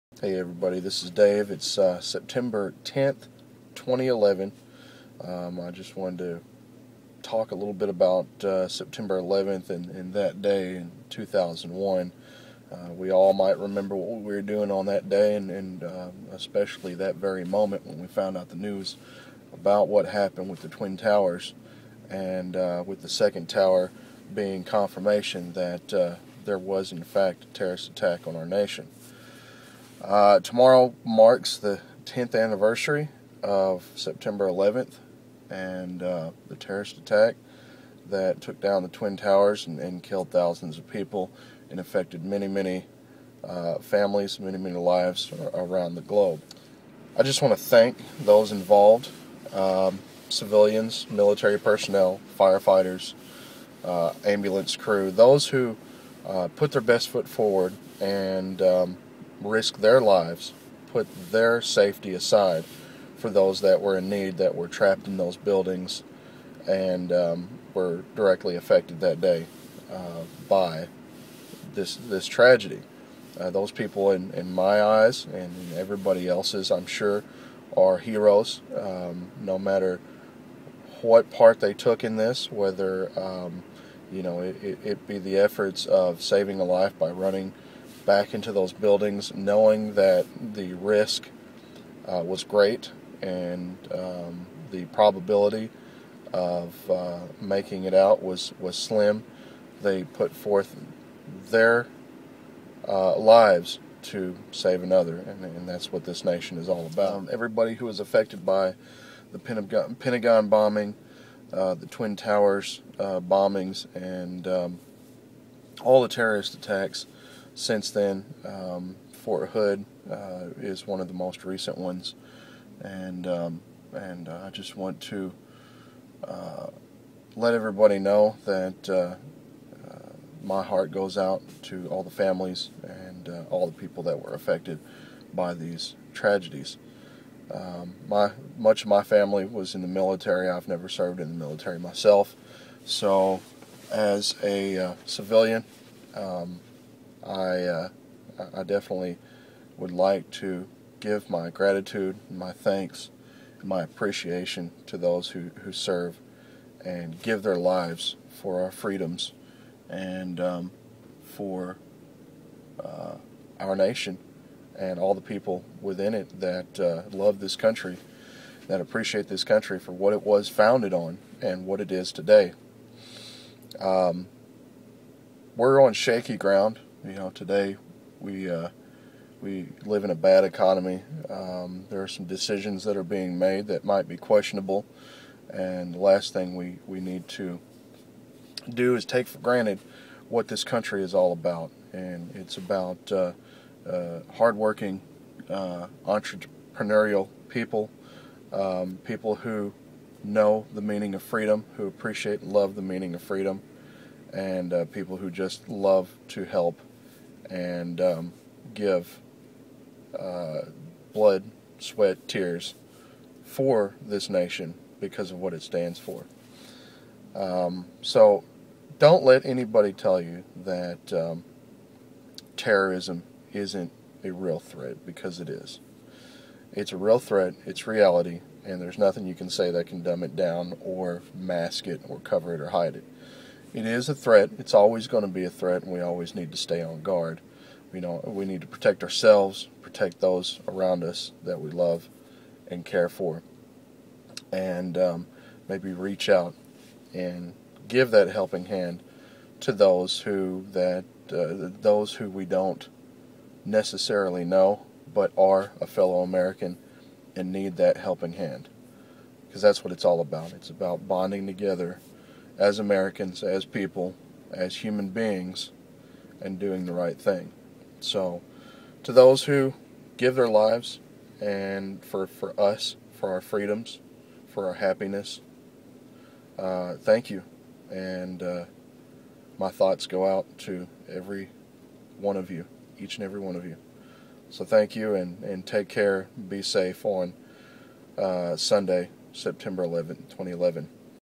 September 11 - 10th Anniversary
September 11, 2011 marks the 10th anniversary of our nations worst tragedy. Thank you to those that have risked their lives for our freedom and safety.